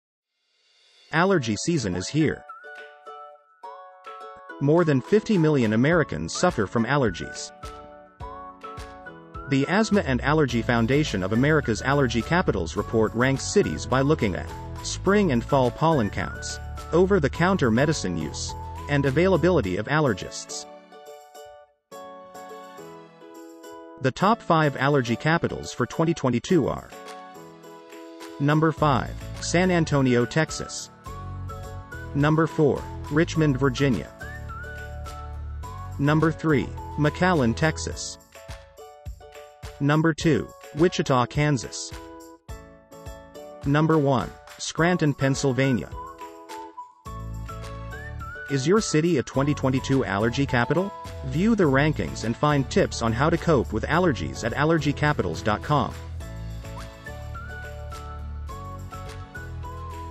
2022 Allergy Capitals Top 5 Cities
It’s spring allergy season! Find out where your city ranks in AAFA’s 2022 Allergy Capitals™️ report and get tips for how to better manage your spring allergies

More than 50 million Americans live with various types of allergies every year. Many of them have seasonal pollen allergies. AAFA’s yearly Allergy Capitals™️ report explores how challenging it is to live with spring or fall allergies in the top 100 U.S. cities*.

The report looks at these important factors:

 - Spring pollen scores
 - Fall pollen scores
 - Over-the-counter medicine use
 - Availability of board-certified allergists/immunologists

In the report, you will also find actions you can take to protect yourself from pollen during outdoor activities as well as in your home.